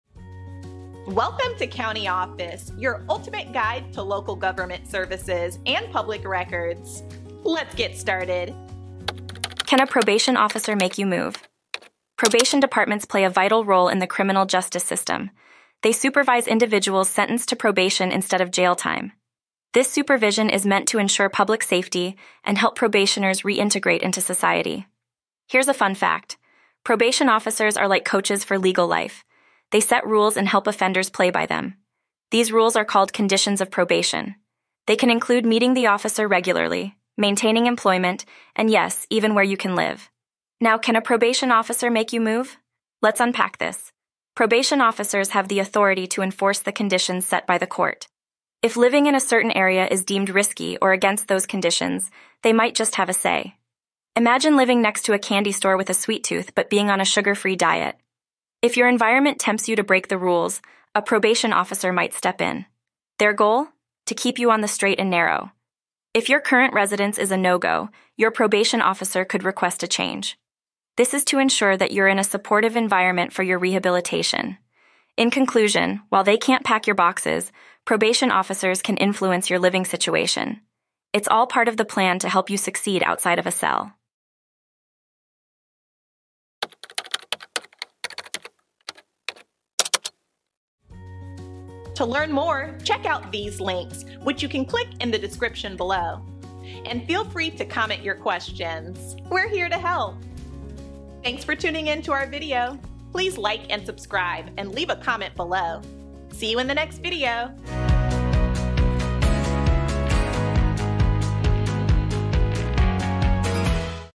Can A Probation Officer Make You Move? - CountyOffice.org
Can A Probation Officer Make You Move? Navigating the conditions of probation can be complex, especially when it comes to understanding the role of a probation officer in your life decisions. In our latest video, we delve into the responsibilities of probation officers and how they might impact where you live during your probation period. Discover the extent of their authority and the reasons behind their decisions to ensure a supportive environment for rehabilitation. Join us as we explore the balance between legal supervision and personal freedom, and what it means for those on probation.